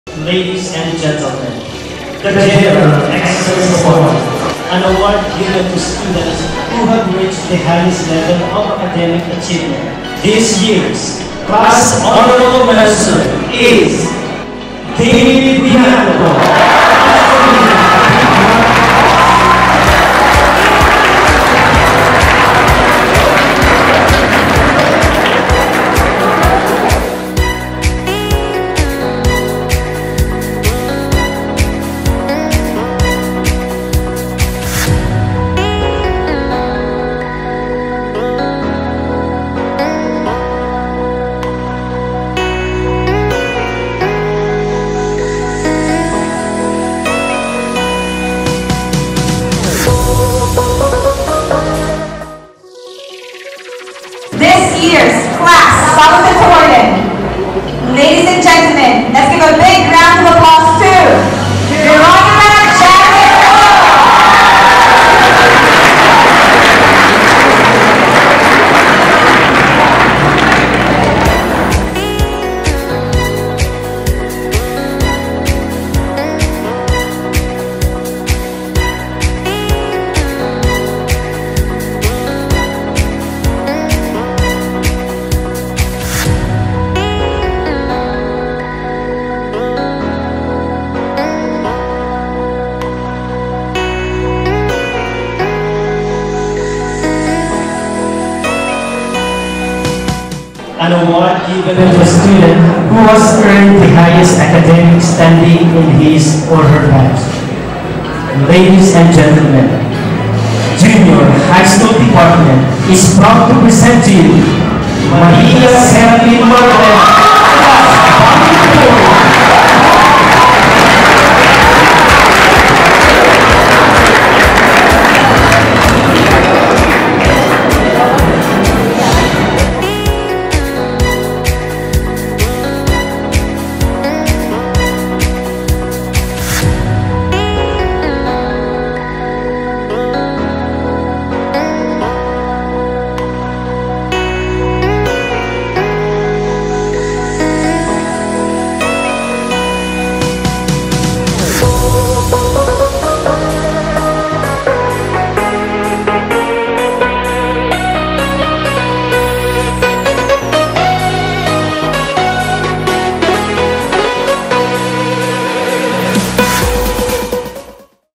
JHS Graduation Day 2019 awarding General Excellence awards
Awarding to best three of graduates.